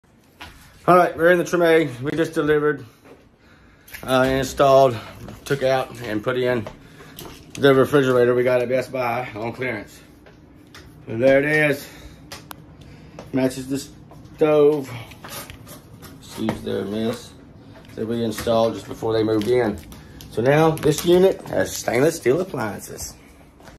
Remove old fridge and installed new Clearance fridge
New fridge for Treme property